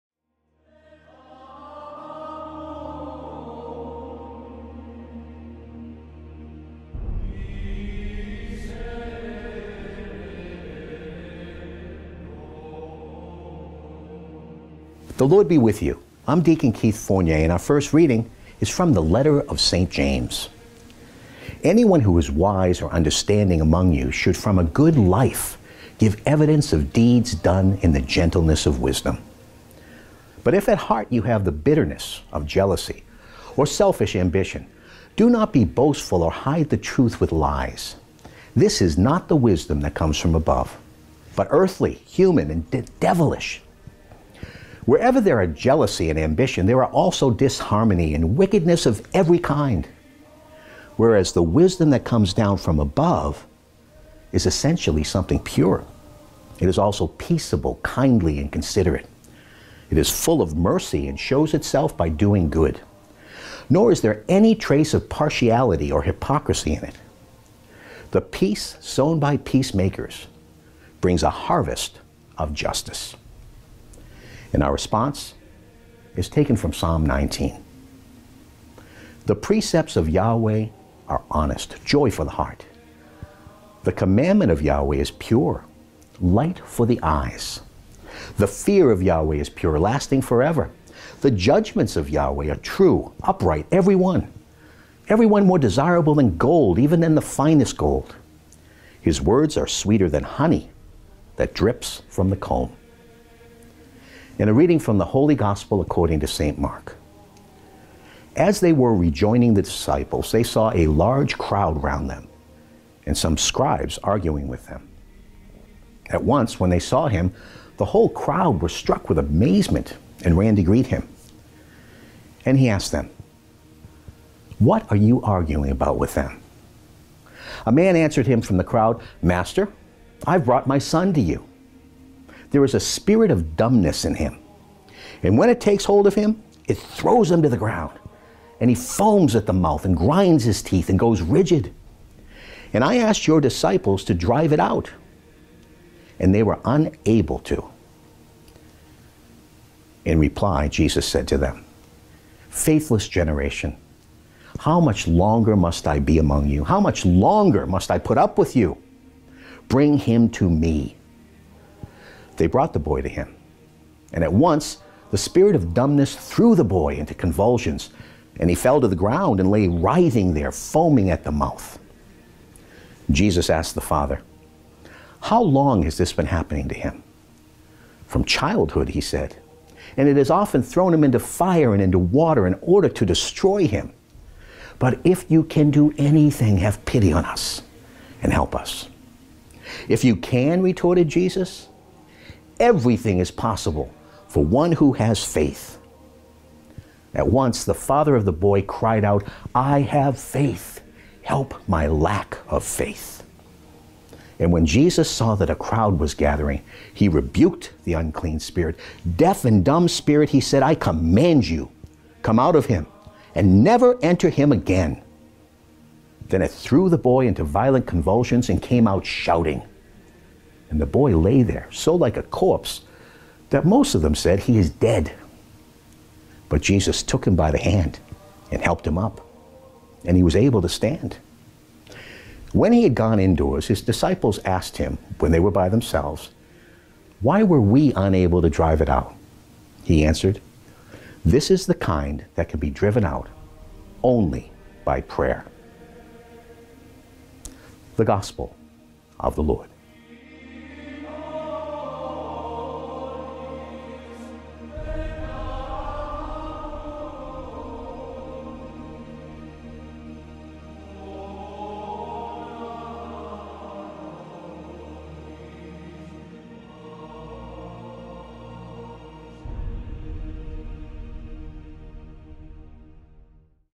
Daily Reading for Monday May 20, 2024 HD
Reading 1, James 3:13-18
Responsorial Psalm, Psalms 19:8, 9, 10, 15
Gospel, Mark 9:14-29